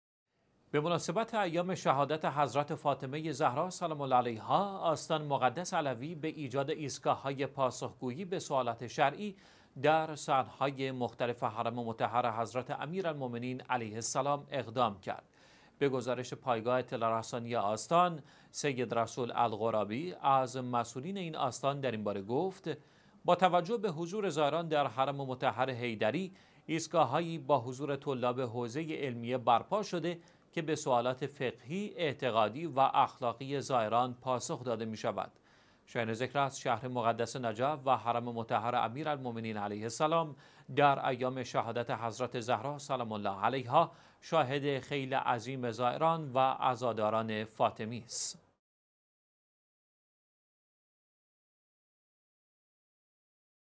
برپایی ایستگاه های پاسخ گویی به مسائل شرعی در آستان مقدس علوی
به مناسبت ایام شهادت حضرت فاطمه زهرا سلام الله علیها، آستان مقدس علوی به ایجاد ایستگاه های پاسخ گویی به سؤالات شرعی، در صحن های مختلف حرم مطهر حضرت امیرالمؤمنین علیه السلام، اقدام کرد.
به گزارش پایگاه اطلاع رسانی آستان، «سید رسول الغرابی» از مسئولین این آستان در این باره گفت: با توجه به حضور زائران در حرم مطهر حیدری، ایستگاه هایی با حضور طلاب حوزه علمیه برپا شده که به سؤالات فقهی، اعتقادی و اخلاقی زائران پاسخ داده می شود.
شایان ذکر است، شهر مقدس نجف و حرم مطهر امیرالمومنین علیه السلام، در ایام شهادت حضرت زهرا سلام الله علیها، شاهد خیل عظیم  زائران و عزاداران فاطمی است.